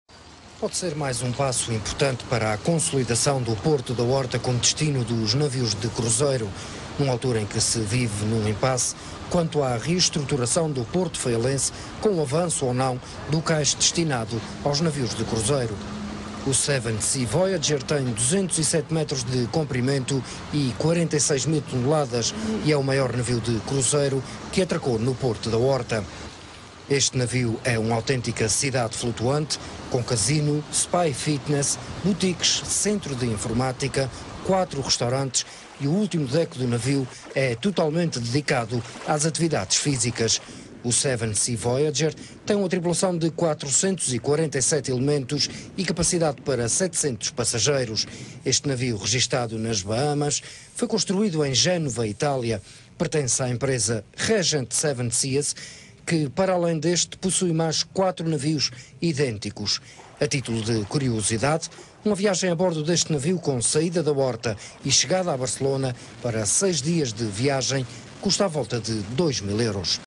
Um dos maiores paquetes do Mundo na Horta - RTP Açores
RTP Açores - 09/05/08 - O Seven Seas Voyager, um dos maiores navios de cruzeiro do Mundo, esteve no porto da Horta.